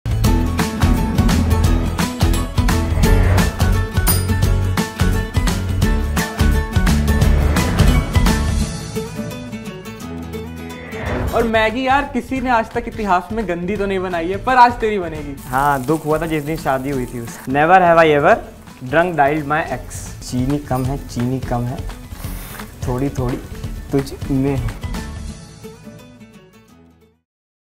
HT Kitchen HappyCooking: Stay tuned to watch Pranshu & Nitish struggle to save their dishes
[PARTNERED]

Students Pranshu & Nitish take the ultimate cooking challenge over a twisted game of Never Have I Ever! Try to cook your signature dish while every time you agree to a statement read by your opponent, they add ANYTHING they want to your dish.

Will their dishes survive the challenge? Or will they end in disaster?